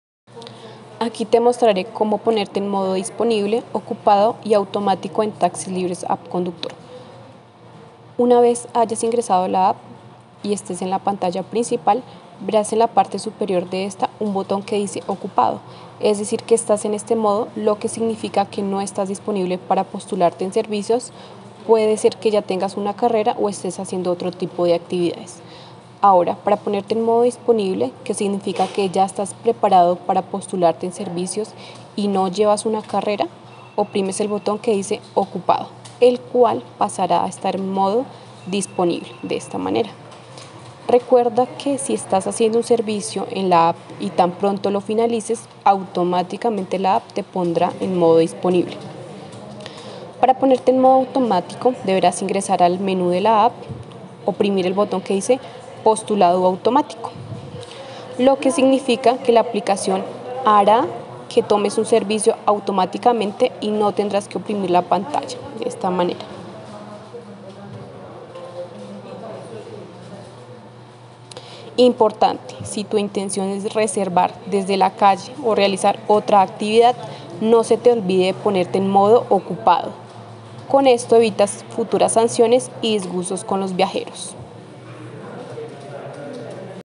¿Cómo ponerme en modo disponible, ocupado y automático en Taxis Libres App Conductor?
Aquí te mostrare como ponerte en modo disponible, ocupado y automático en Taxis Libres App Conductor.

Una vez hayas ingresado a la app y estés en la pantalla principal, verás en la parte superior de esta un botón que dice Ocupado, es decir que estás en este modo, lo que significa que no estás disponible para postularte en servicios, puede ser que ya tengas una carrera, o estés haciendo otro tipo de actividades.

Ahora, para ponerte en modo Disponible, que significa que ya estás preparado para postularte en servicios y no llevas una carrera, oprimes el botón que dice Ocupado el cual pasará a estar en modo Disponible, de esta manera.

Recuerda que si estás haciendo un servicio en la app, y tan pronto lo finalices, automáticamente la app te pondrá en Modo Disponible.

Para ponerte en Modo Automático, deberás ingresar al menú de la app, oprimir el botón que dice: Postulado automático, lo que significa que la aplicación hará que tomes un servicio automáticamente y no tendrás que oprimir la pantalla. De esta manera.

Importante, si tu intención es reservar desde de la calle o realizar otra actividad, no se te olvide ponerte en Modo Ocupado, con esto evitas futuras sanciones y disgustos con los viajeros.